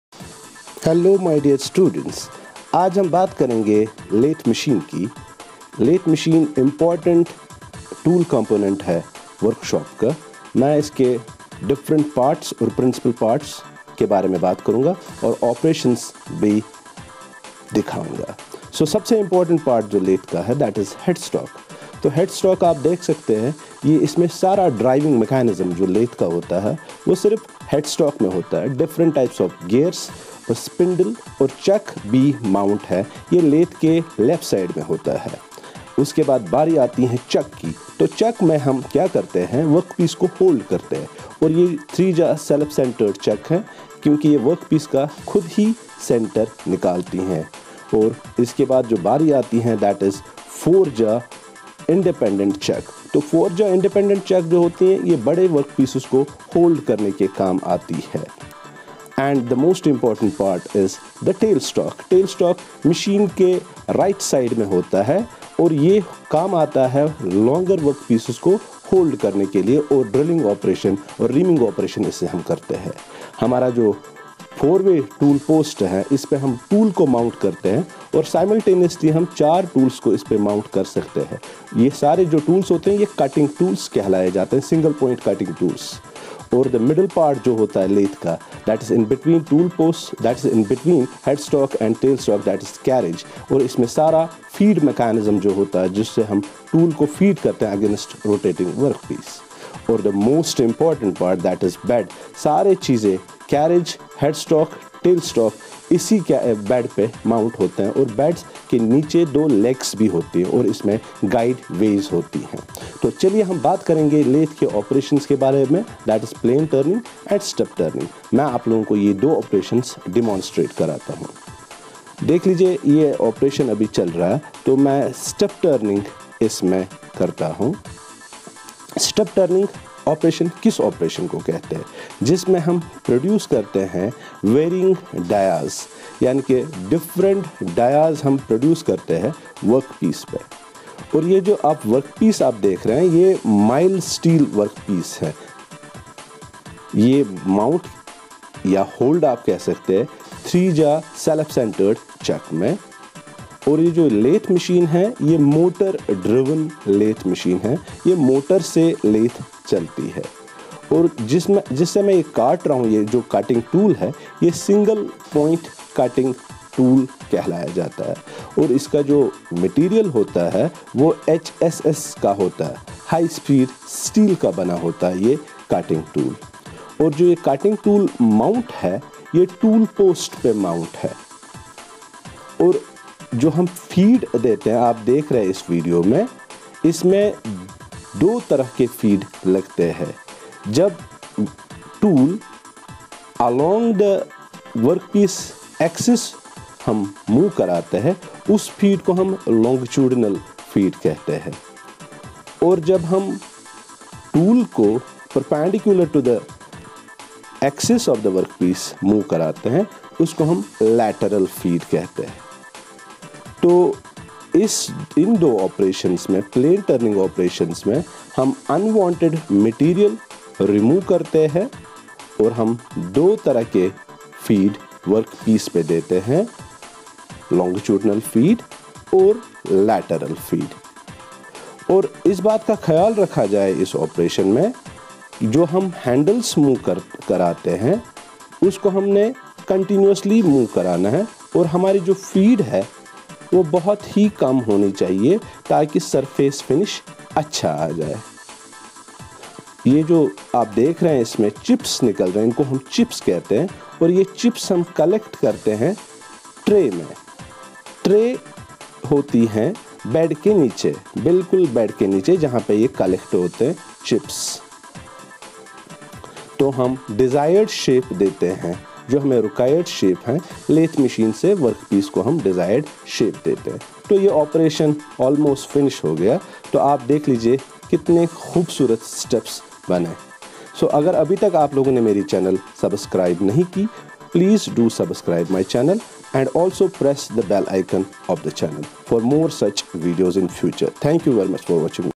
Parts of Lathe || Plain Turning || Demonstration
lathe principal parts and two important operations demonstrated in this video.